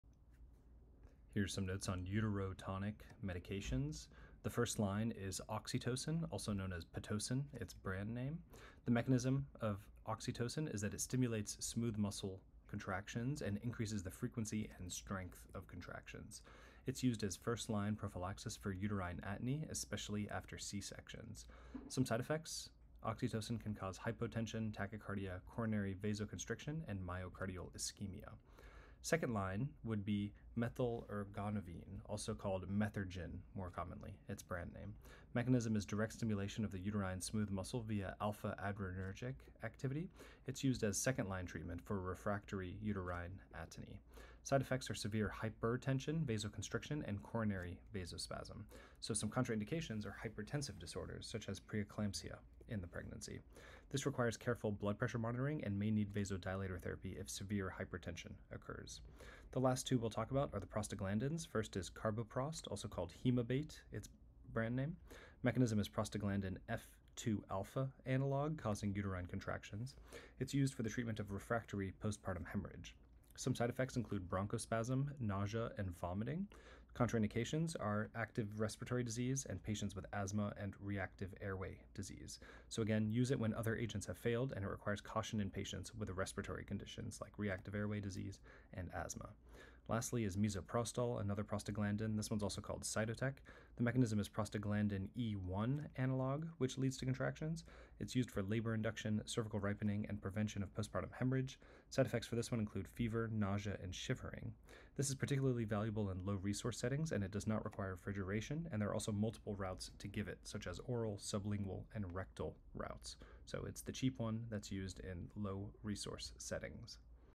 Uterotonic medications
First line: Oxytocin (Pitocin®, Syntocinon®)
- **Mechanism**: Stimulates uterine smooth muscle contractions and increases frequency/strength of contractions
- First-line prophylaxis for uterine atony, especially post-cesarean
- **Side Effects**: Hypotension, Tachycardia, Coronary vasoconstriction, Myocardial ischemia

Second-line: Methylergonovine (Methergine®)
- **Mechanism**: Direct stimulation of uterine smooth muscle via alpha-adrenergic activity
- Second-line treatment for refractory uterine atony
- **Side Effects**: Severe hypertension, Vasoconstriction, Coronary vasospasm
- **Contraindications**: Hypertensive disorders, Preeclampsia
- **Clinical Notes**: Requires careful BP monitoring; may need vasodilator therapy if severe htn occurs

Carboprost (Hemabate®, Prostodin®)
- **Mechanism**: Prostaglandin F2α analog causing uterine contractions
- tx of refractory postpartum hemorrhage
- **Side Effects**: Bronchospasm, Nausea, Vomiting
- **Contraindications**: Active respiratory dz, Asthma / RAD
- **Clinical Notes**: Used when other agents have failed; requires caution in pts w respiratory conditions

Misoprostol (Cytotec®)
- **Mechanism**: Prostaglandin E1 analog →  contractions
- Labor induction, cervical ripening, prevention of postpartum hemorrhage
- **Side Effects**: Fever, Nausea, Shivering
- **Clinical Notes**: Particularly valuable in low-resource settings; Does not require refrigeration; Multiple routes (oral, sublingual, rectal)